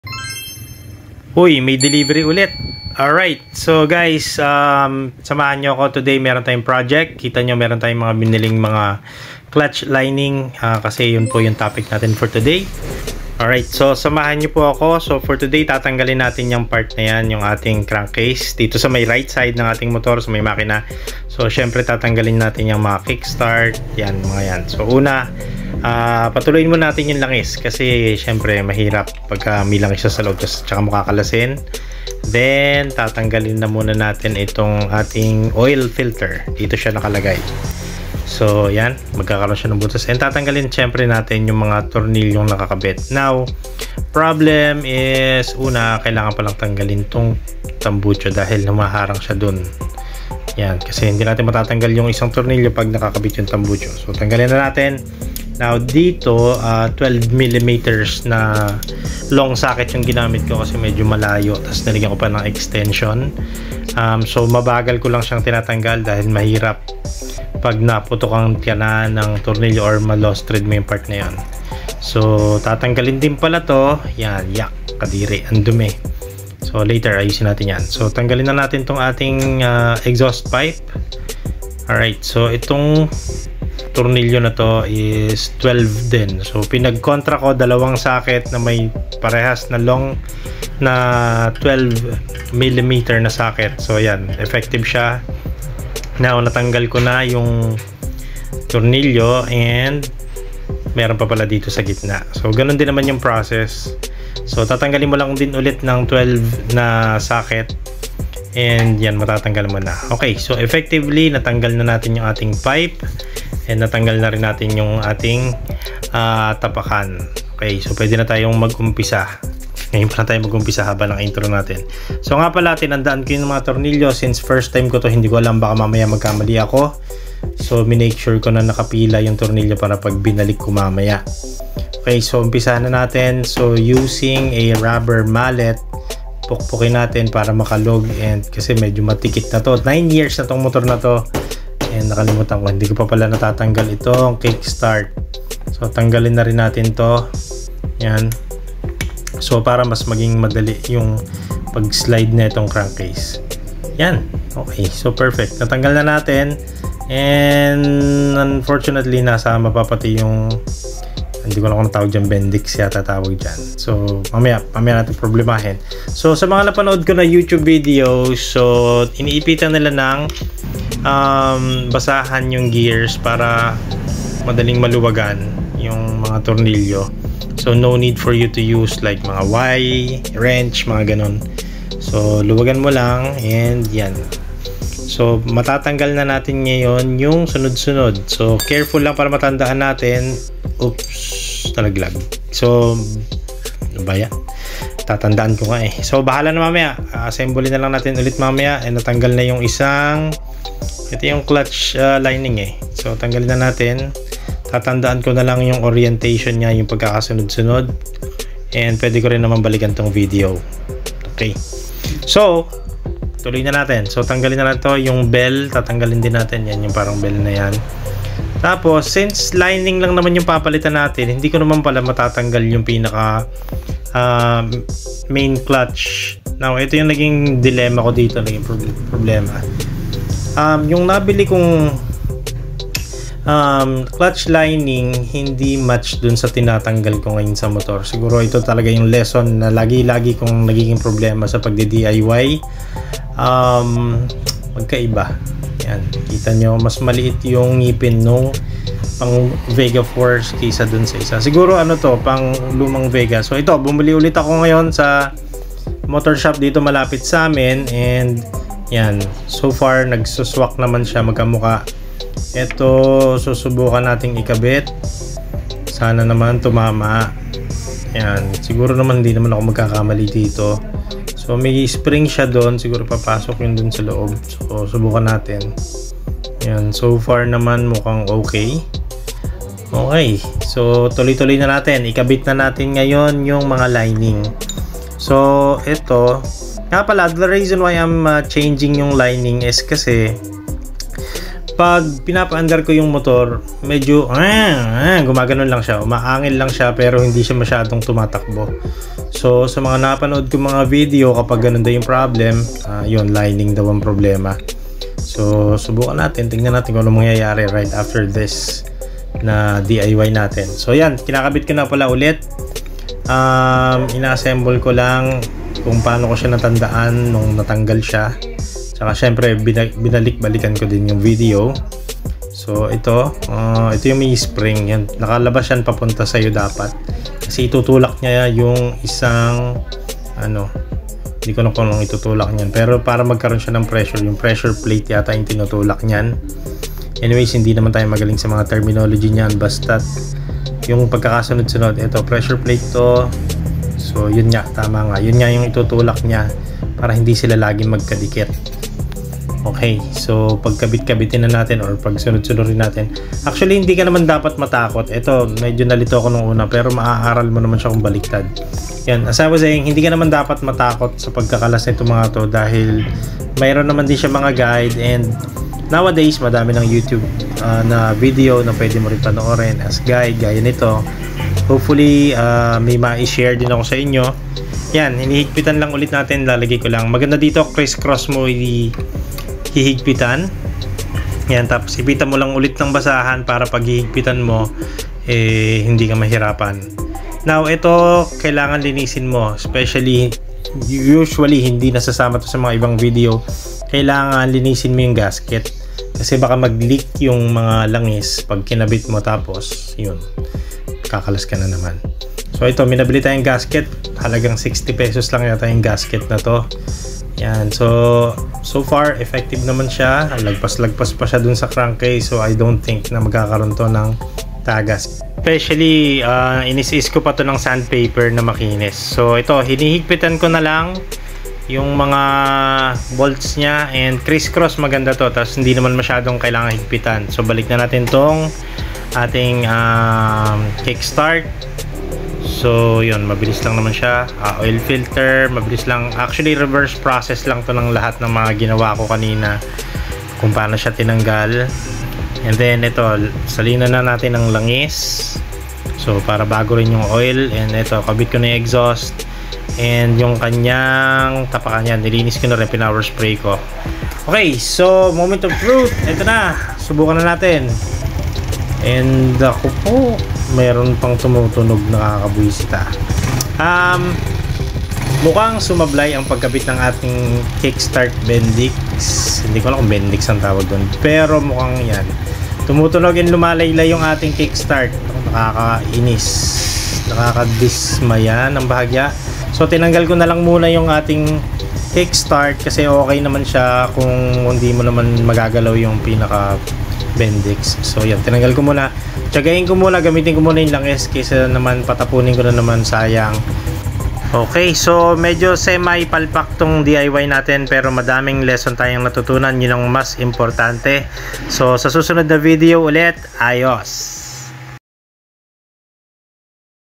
Clutch Lining Replacement-Yamaha Vega Force fi
Let me share my experience changing the clutch lining of my motorcycle. This was my first time so I did a couple of mistakes along the way but still, for me it was worth trying because I have learned a lot going thru the process. thanks for the support!